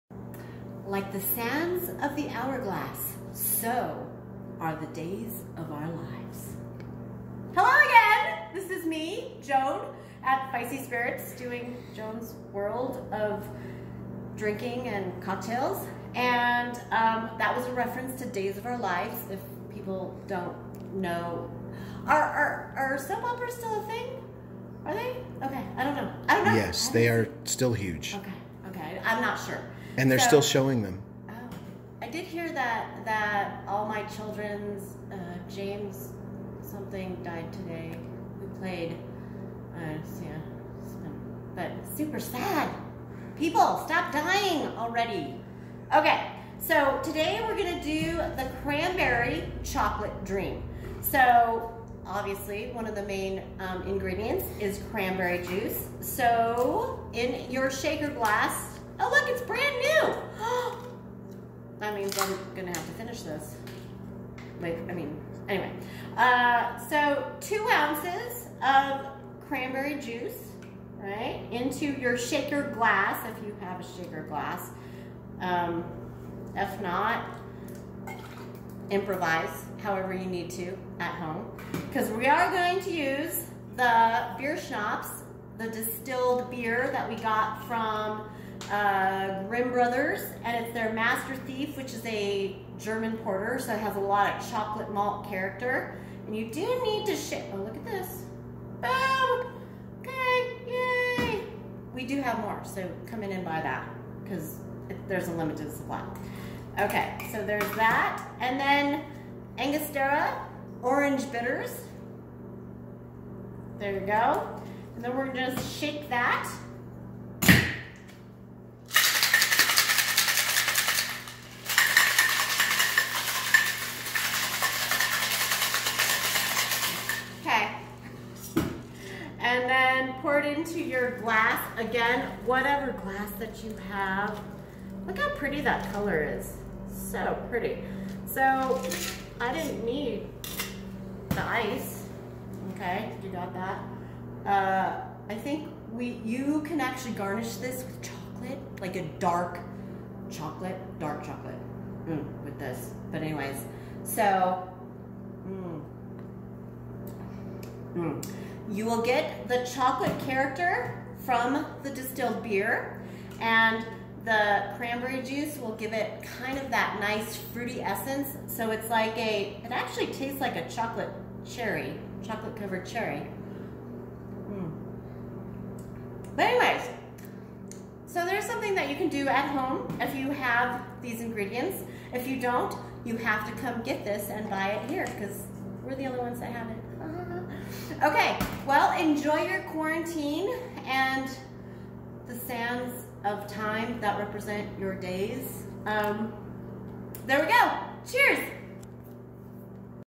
Cranberry Chocolate Dream
Cranberry juice, Distilled Grimm Brothers Master Thief German Porter, orange bitters